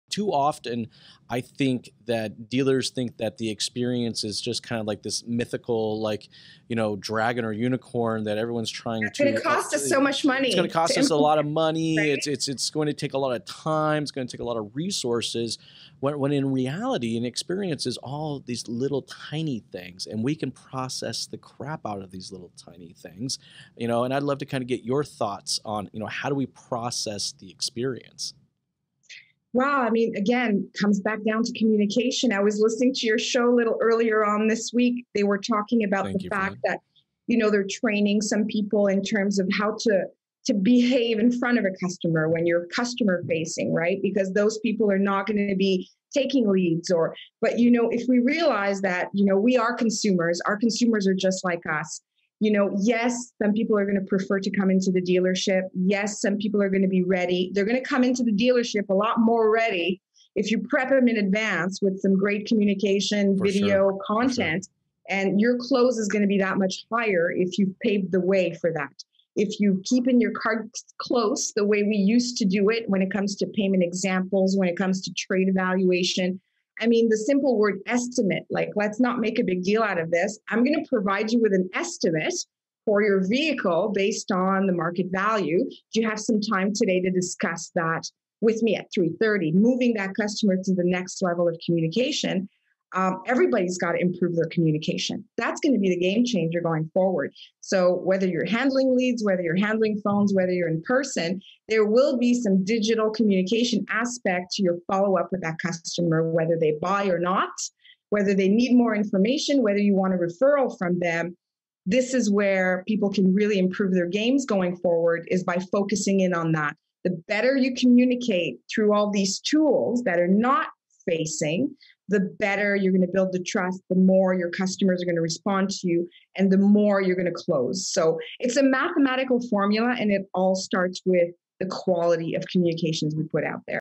Strategy Mob Podcast Ep 90 - Carla Ksonzek - How Do you Process Experience?
Carla talks about how processing experience all comes down to communication and if you prep the customer in advance with the various digital tools at your disposal you just increase your chances of success.